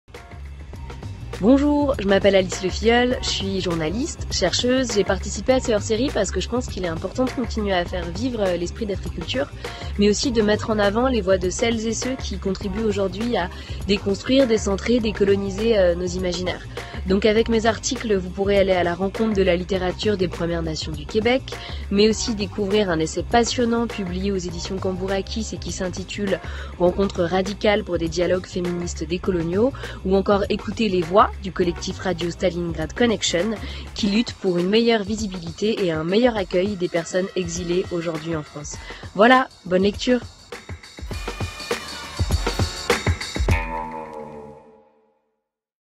Alice Lefilleul, journaliste et chercheuse recommande le hors-série Africultures
La journaliste et chercheuse Alice Lefilleul nous explique pourquoi il faut commander le hors-série Africultures.